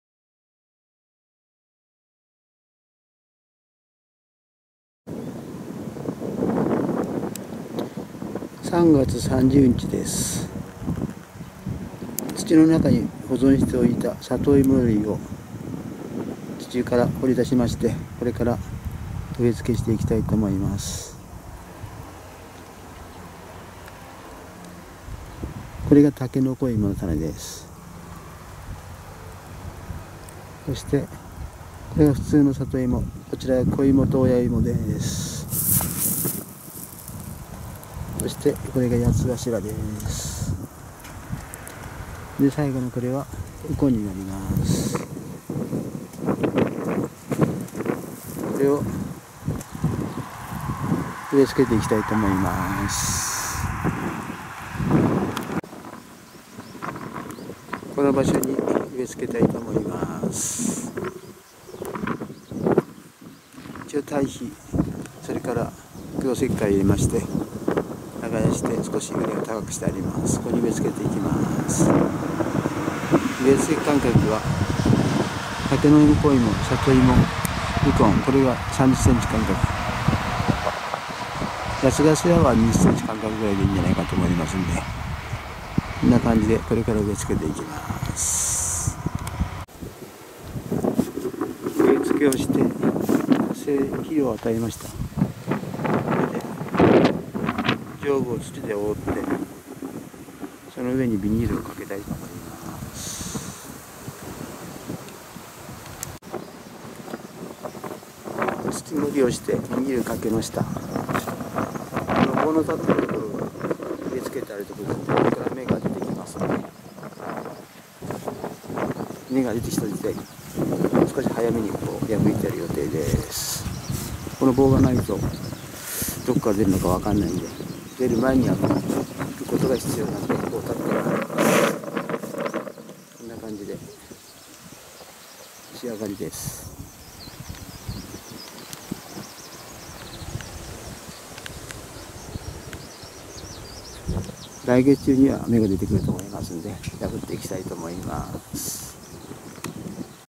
シニアおじさんのスローライフ#291（サトイモの類植付をする）
３月３０日
屋外にサトイモ類の植付をして、ビニルで覆い発芽を待つ